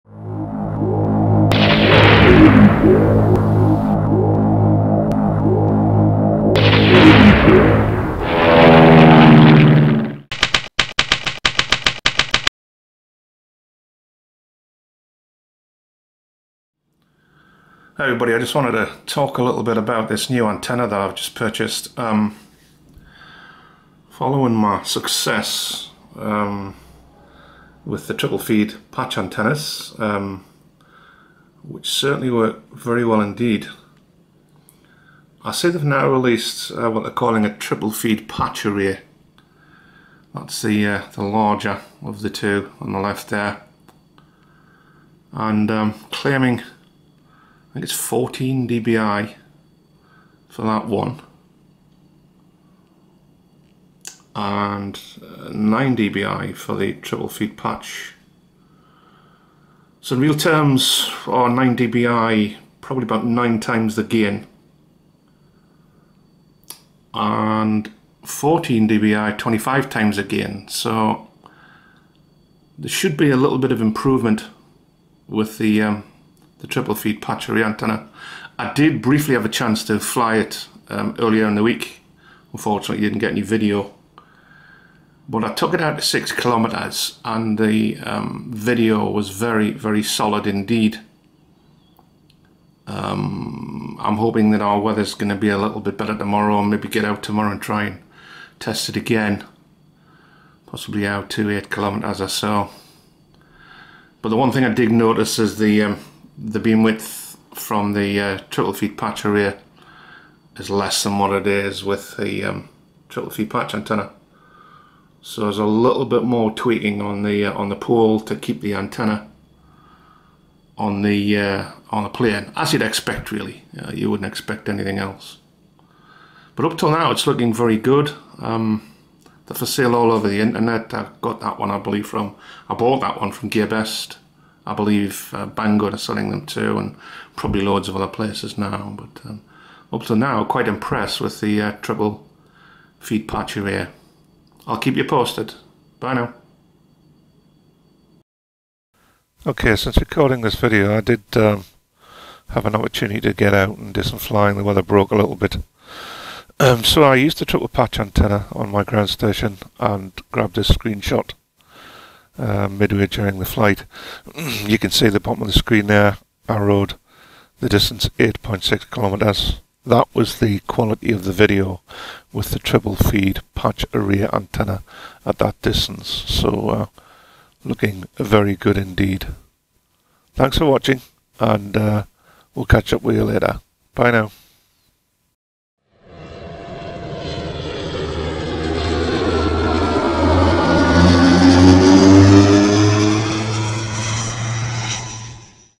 8.6kms on Triple feed Patch Array 5.8ghz Antenna.
Bought myself one of these to run some tests after having some good success with the Triple Feed Patch. It is working well after a short test.  Beamwidth is slightly less than the smaller alternative but it has more gain.